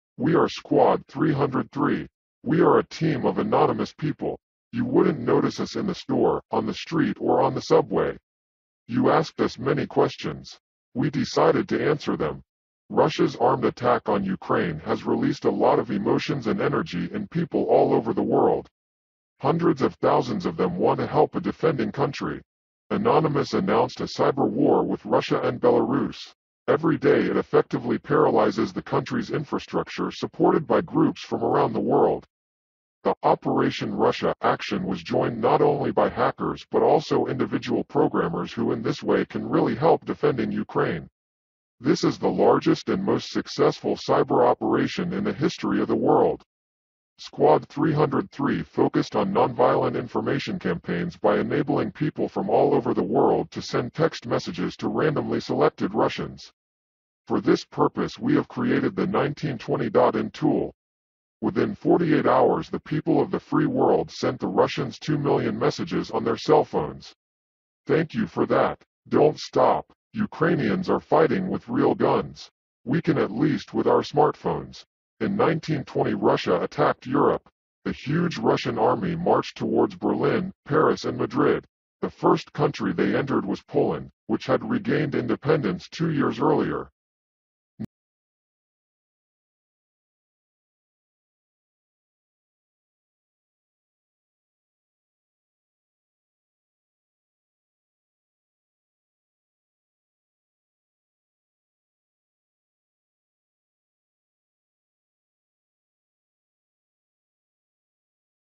Squad 303 OpRussia
Greetings citizens of the world. We are Squad 303.

When Russia invaded Ukraine, we declared cyberwar against Government of Russia and to man himself, Vladimir Putin.

It has indeed had some effect on them, but now we’ll have to prepare for the last push. He isn’t giving up on taking over Ukraine. We must hit them so hard, that it’ll paralyze their whole system. Putin cannot end the war in Ukraine until he has annexed it, or he is forced to. And NATO and EU won’t be the ones stopping it.

    We have seen that already.
    So now, we must act.
    We must stop this war by any means necessary.

10 years ago, Time listed "Anonymous" as one of the "100 most influential people" in the world. Now, we are bigger and stronger than ever before. We must show our true power to Russian government and their president Vladimir Putin. We must unite or else there might be a day that it’s too late to do it. Putin must be stopped, or else we’ll suffer the consequences.